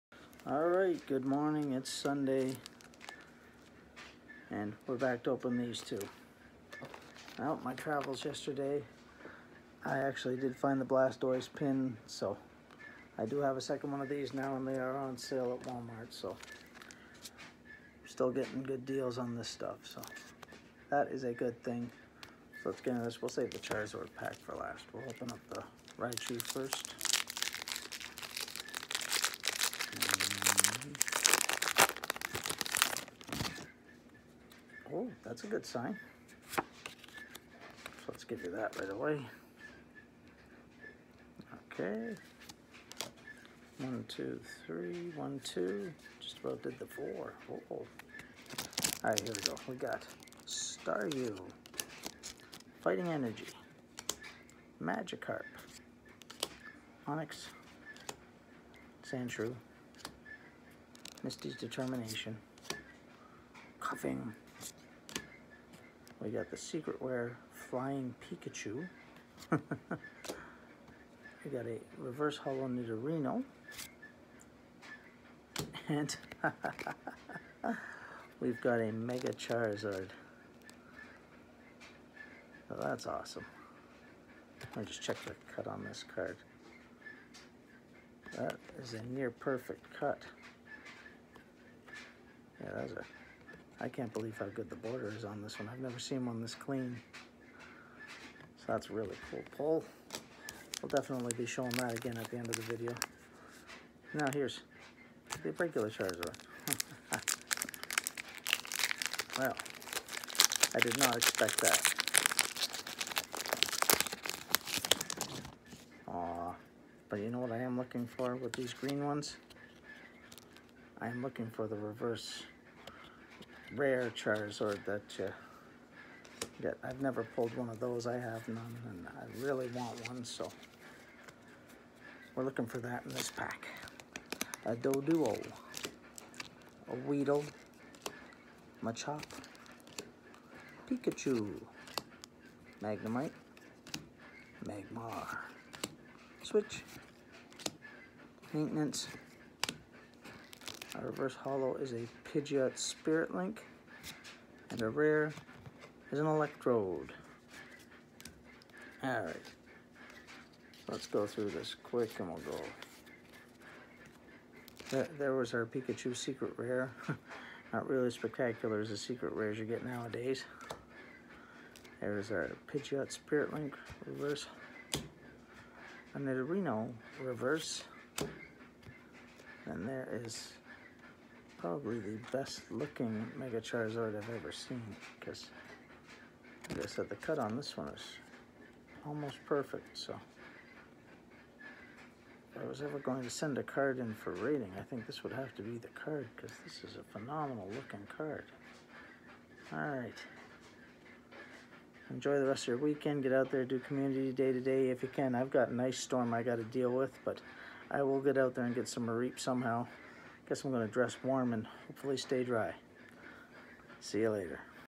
Pokémon Venusaur Pin Evolution Packs!
Here is the “meat and potatoes” of the Venusaur Blister! Ironically what I pull does not have a pin in this series! Enjoy!